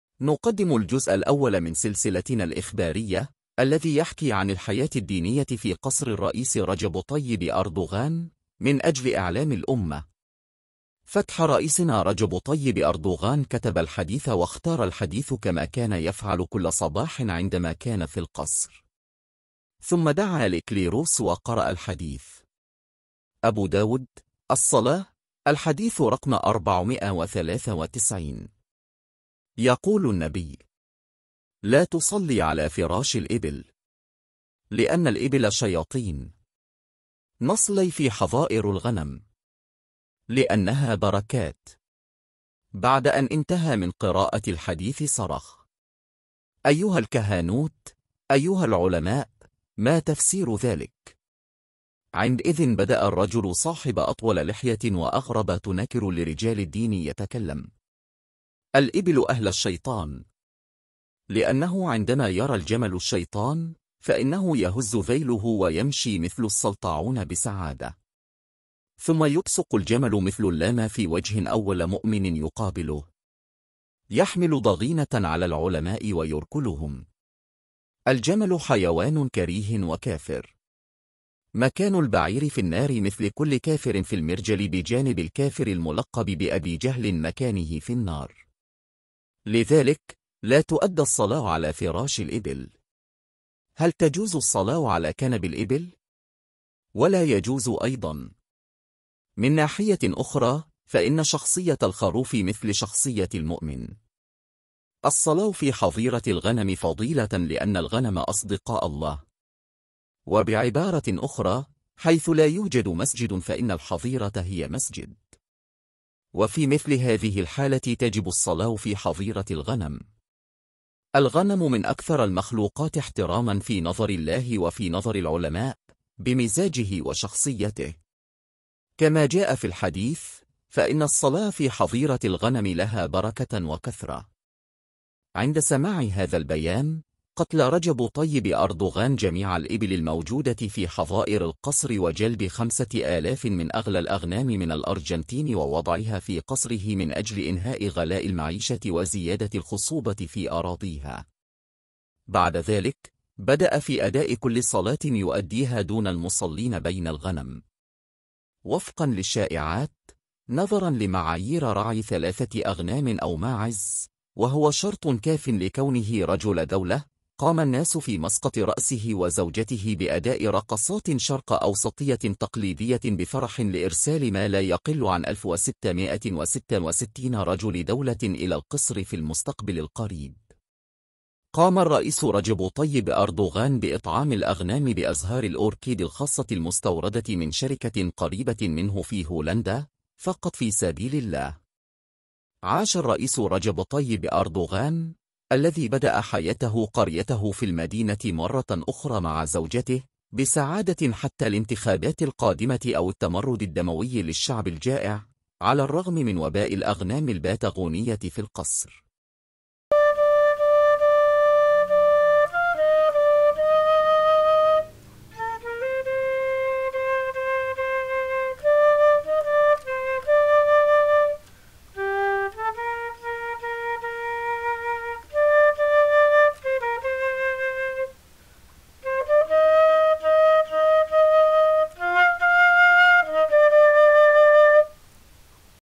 الرئيس رجب طيب أردوغان ، الجمال والأغنام في القصر
تقوى رئيسنا رجب طيب أردوغان معروفة للجميع. نكشف في هذه السلسلة كيف يحاول رجب طيب أردوغان تطبيق كل قاعدة دينية في قصره وفي حياته وكيف يدرس الأحاديث. مصدر الحكم الشرعي في الفيديو أبو داود ، صلاة ، الحديث رقم 493.